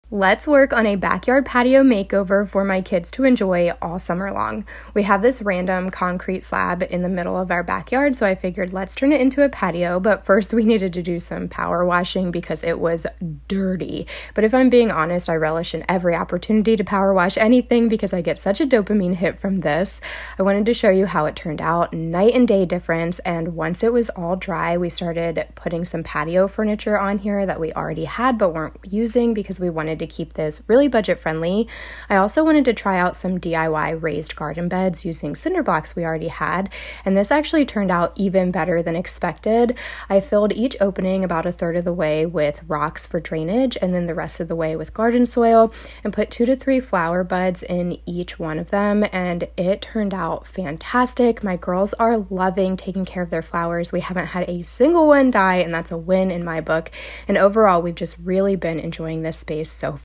Backyard Patio Makeover Ideas
My kids have been loving this backyard lounge, so I thought I’d share these DIY patio makeover ideas for anyone else who happens to have a random concrete slab in the middle of their yard. 😵‍💫